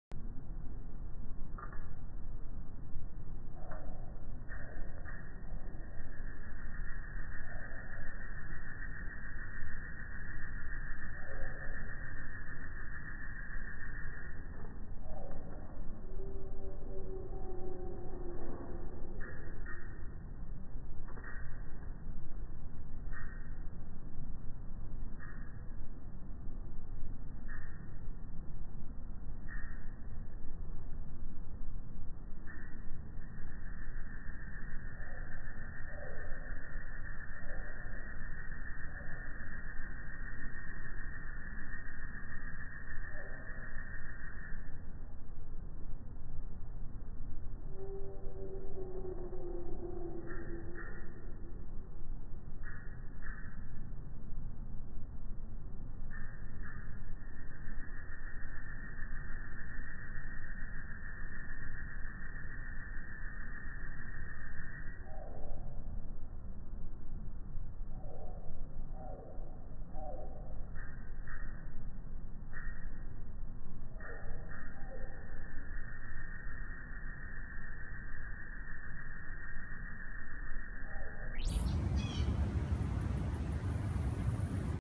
Monarch migration at Texas Discovery Gardens
Slow motion capture of migrating monarchs and bees on frostweed at Texas Discovery Gardens. The high pitched chirping if from cicadas!